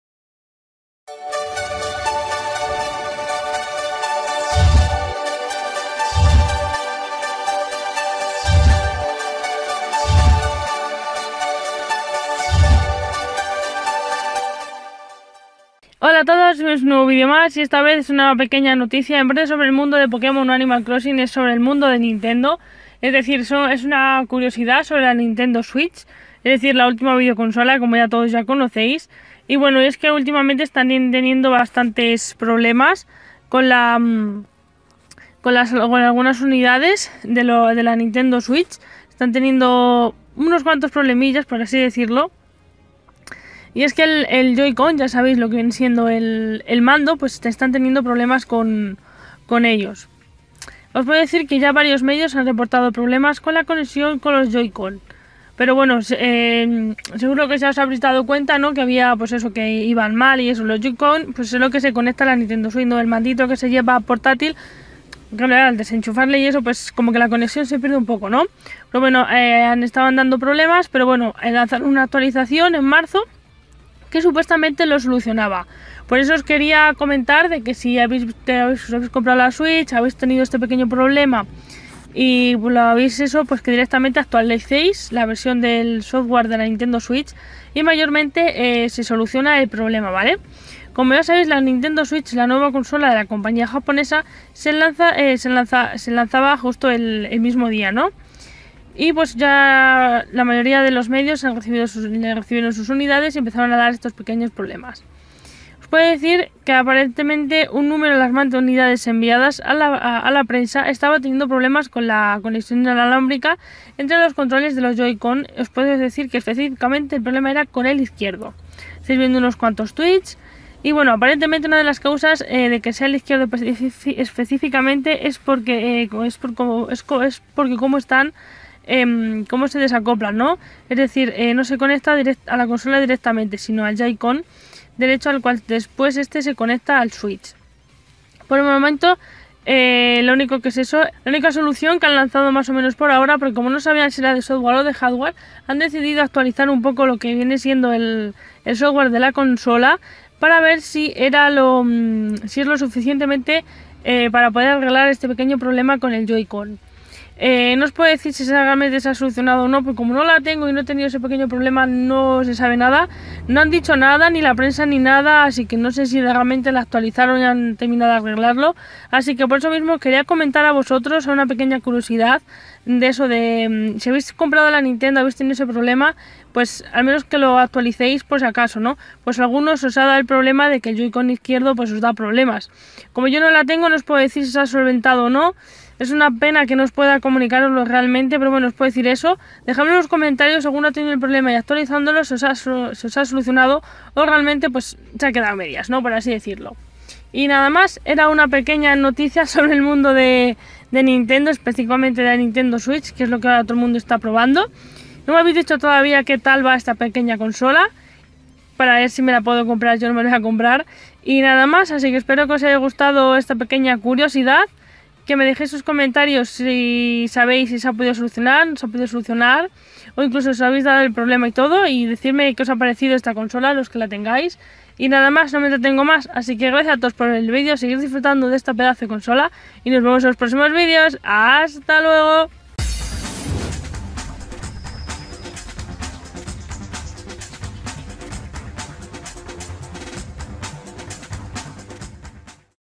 Algunas unidades de Nintendo Switch están teniendo problemas con los Joy Con
Os cuento una curiosidad sobre nintendo switch.